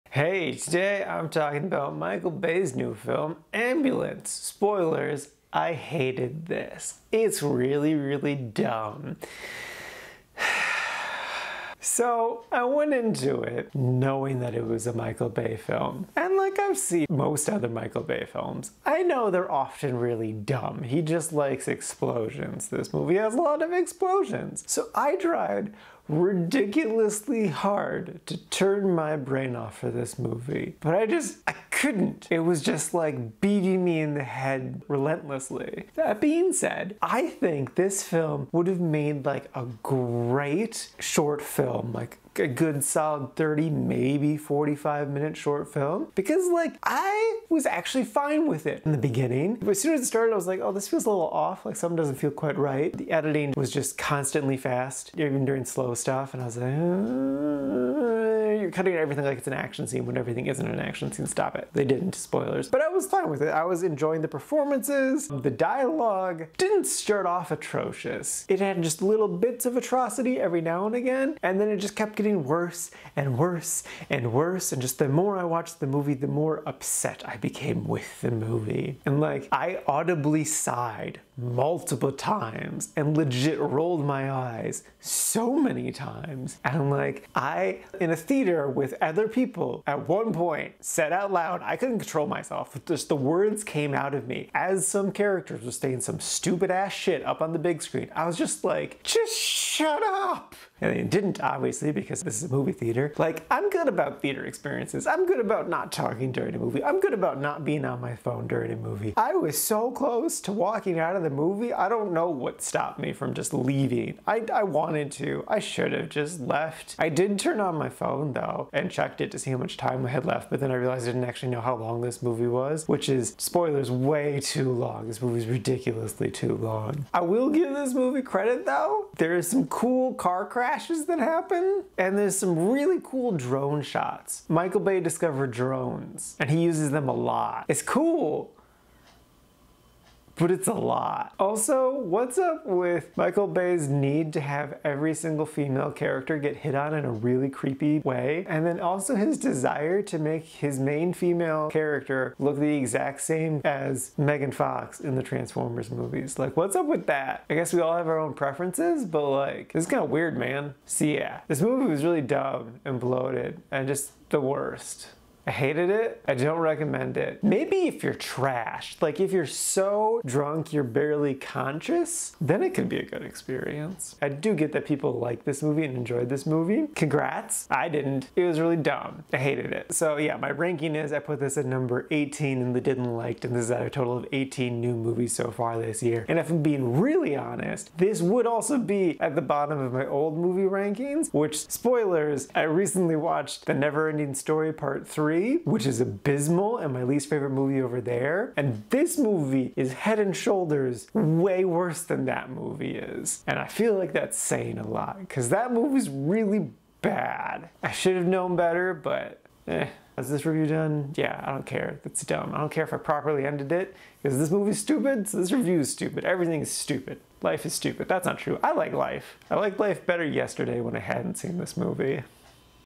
Ambulance - Charlie's Movie Review
I really hated this movie. I'm sorry if you liked it. Though, I'm not really sorry. I really hated this...